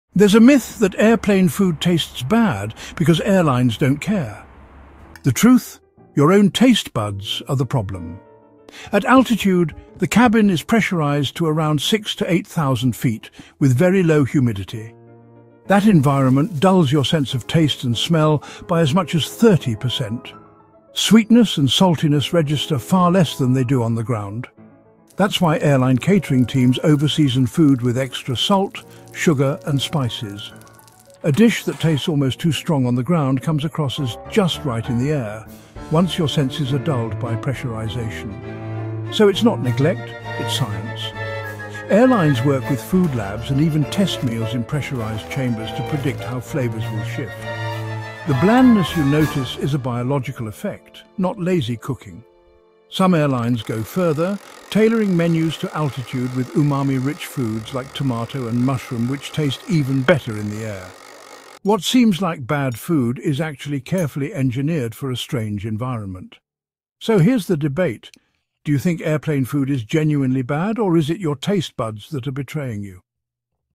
Is Plane Food Really Terrible — or Just a Myth at 30,000 Feet
Airplane Food: Myth vs Truth
Myth: Airline food is just bad by design.
Truth: At 35,000 feet, your taste buds go numb — studies show your sense of salt and sweet can drop by up to 30%. That’s why meals are seasoned more aggressively, with chefs designing recipes to shine at altitude. It’s not neglect — it’s pure science.

So… is airplane food really bland, or is it your taste buds playing tricks on you?